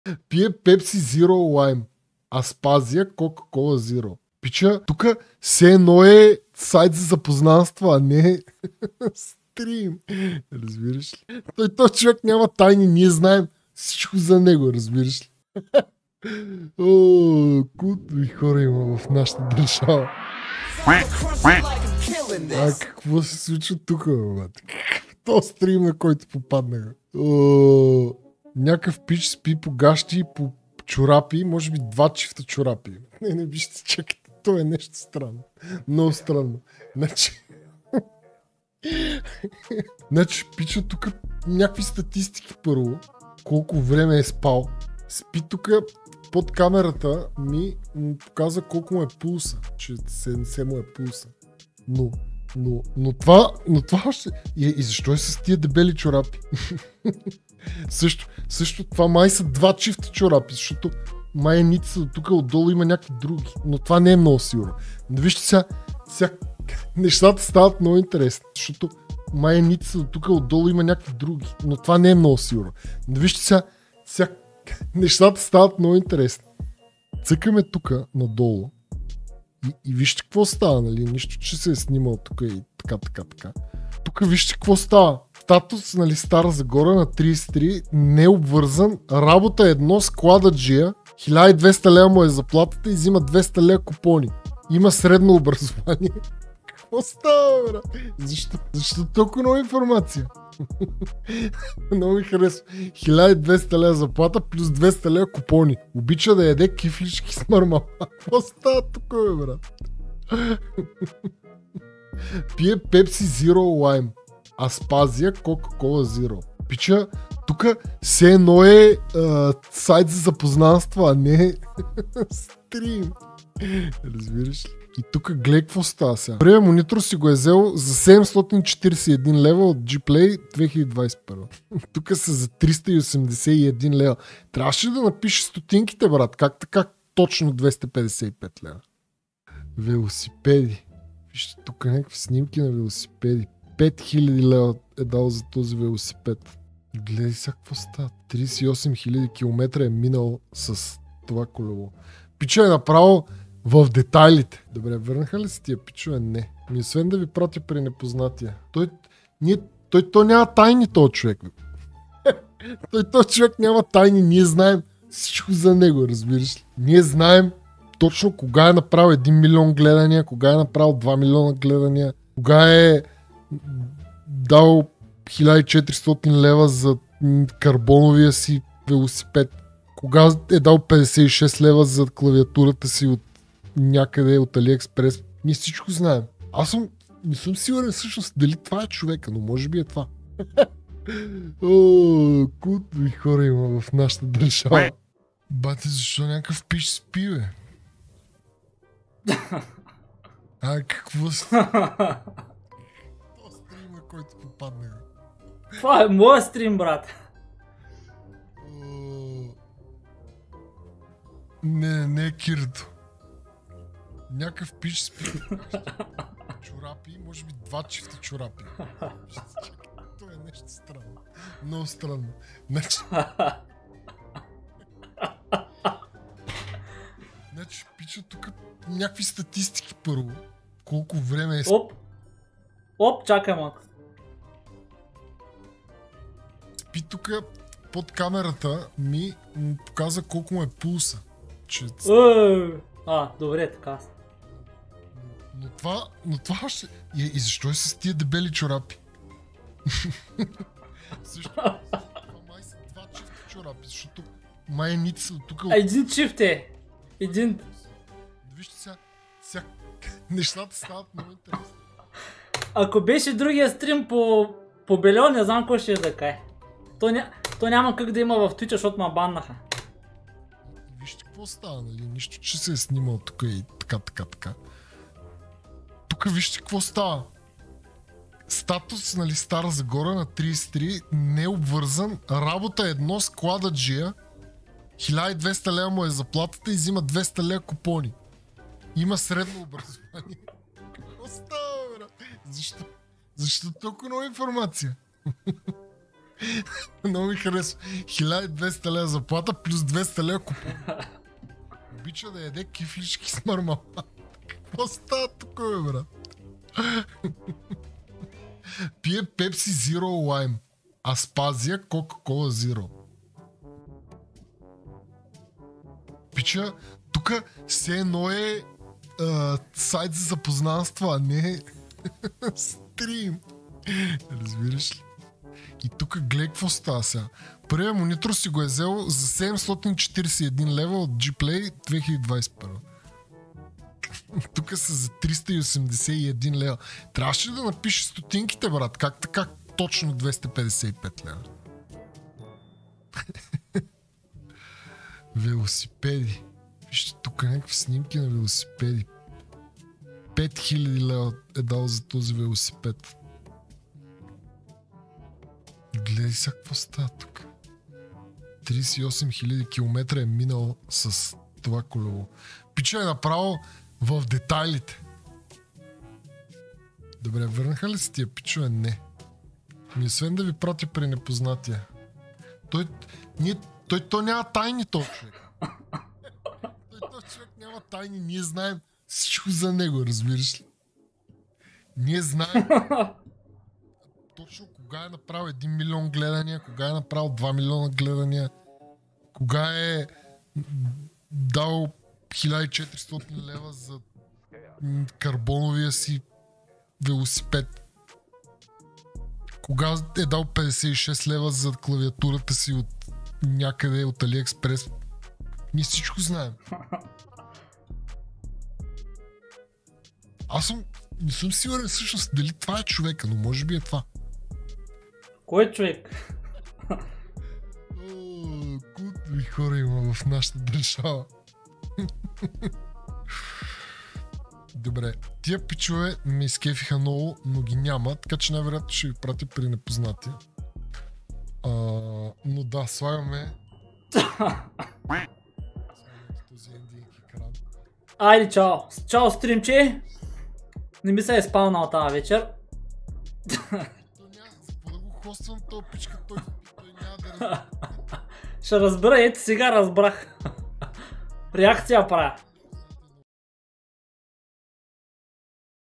попада на един НЕОБИКНОВЕН стрийм... + Реакция
0:00 - NoThx открива необикновенния стрийм
3:16 - Wizhunts реагира на NoThx